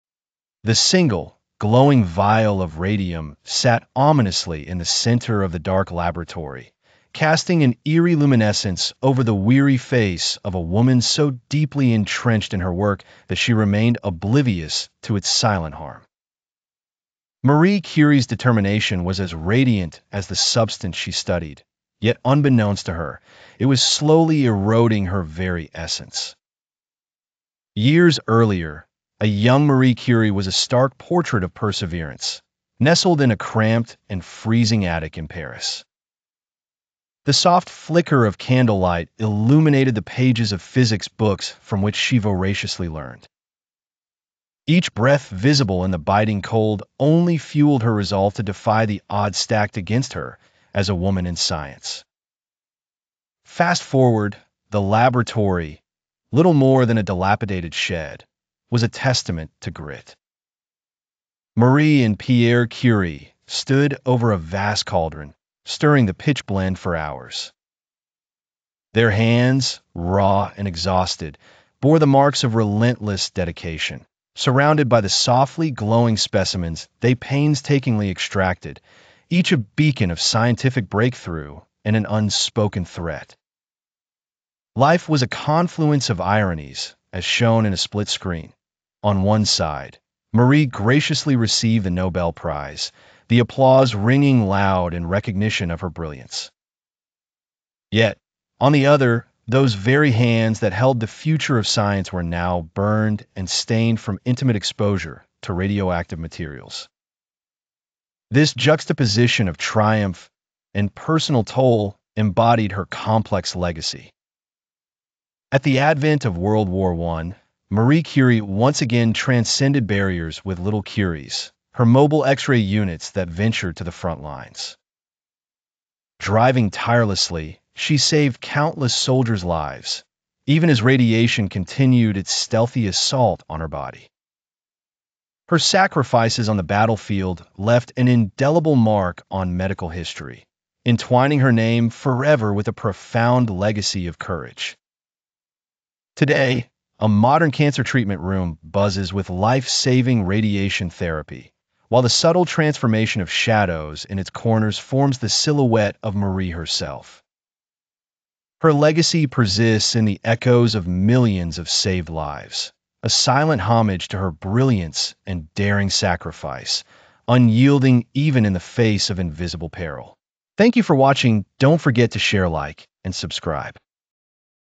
Day 10 - the Glowing Killer She Carried in Her Pocket: Marie Curie's Deadly Secret
She carried tubes of radium in her pocket, not knowing they were burning her skin.
She won two Nobel Prizes,but was rejected from the French Academy... for being a woman.

This is the UNTOLD story of Marie Curie:
☢️How she discovered two elements in a leaky shed
💔The double sacrifice she made for science
🩺How her work in WWI saved countless lives
⚖️The brutal prejudice she faced as a female scientist

She didn't just win Nobel Prizes—she literally gave her life to science, and her discoveries continue to save millions from cancer today.

If you know anyone who has ever had an X-ray or radiation treatment, they owe a debt to this woman's incredible sacrifice.

🌐 Series: 100 Days of Human Genius

💬 Interactive Question:
Was Marie Curie's sacrifice worth it?Did the benefits of her discovery (radiation therapy, X-rays) outweigh the ultimate cost of her own life? Debate below!